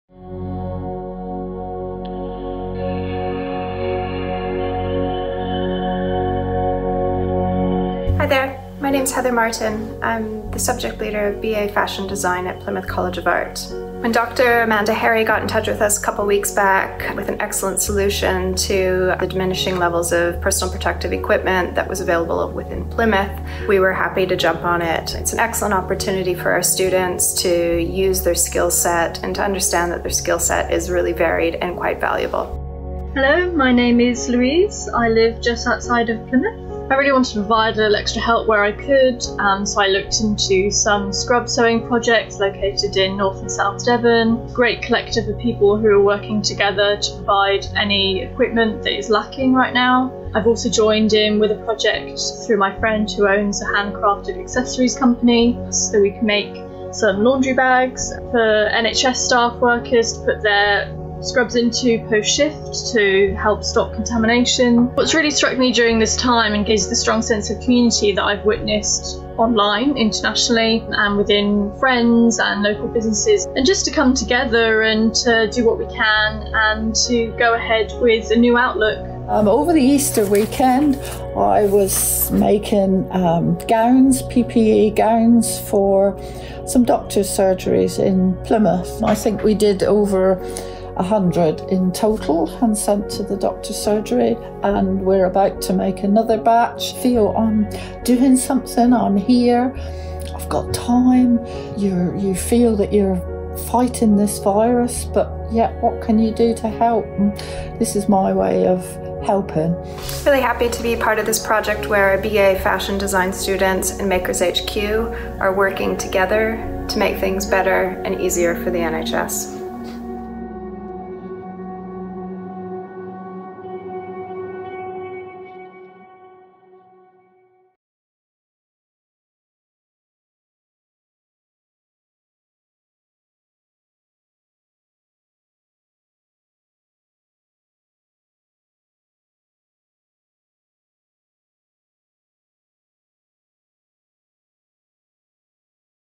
Staff and Students are making scrubs for the NHS
Thank you to all the staff and students of Arts University Plymouth that are using there skill set to help support our NHS staff in these difficult times.